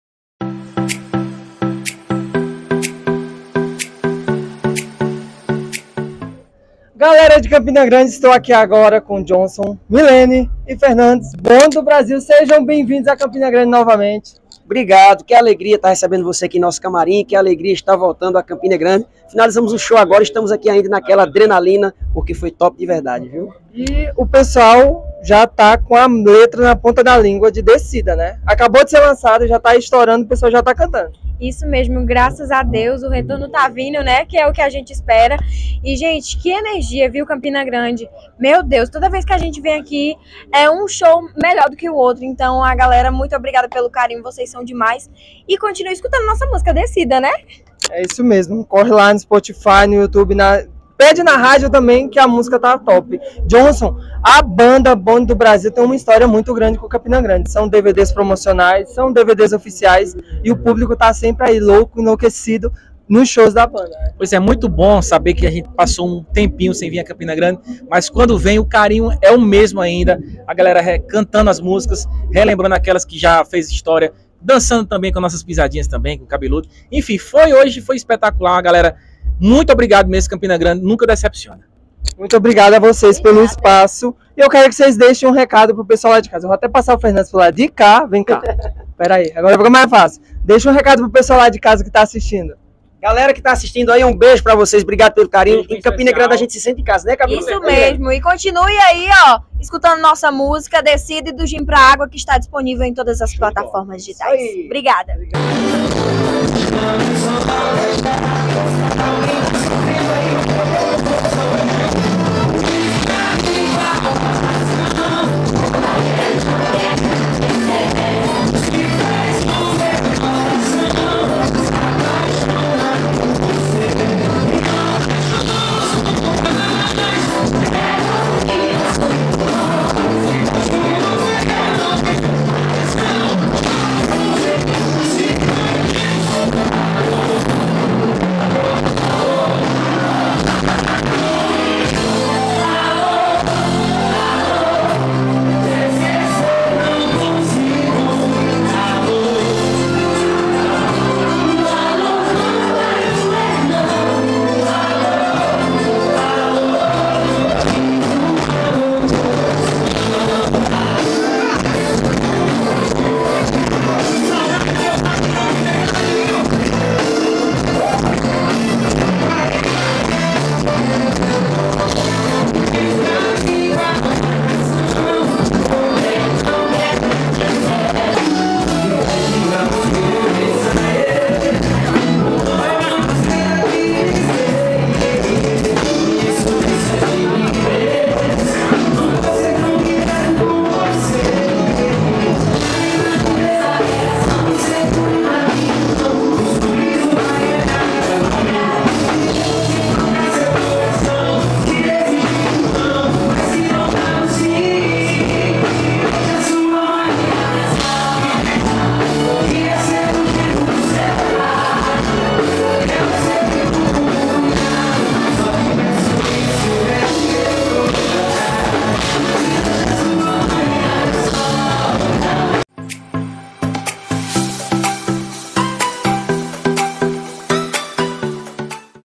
Filipe Mello entrevista Bonde do Brasil no Jardins Clube em Campina Grande/PB
Muito Obrigado!!

Imagens e Edição: SLM Comunicação Visual